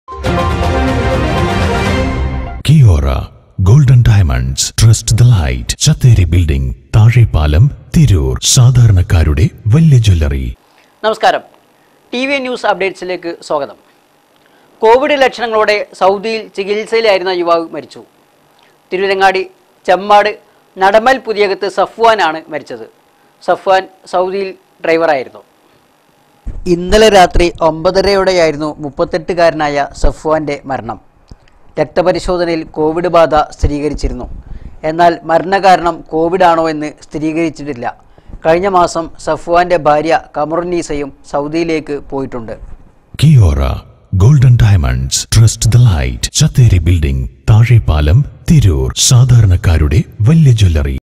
TVN NEWS UPDATES
കോവിഡ്  ബാധിച്ച
ചെമ്മാട് സ്വദേശി  മരിച്ചു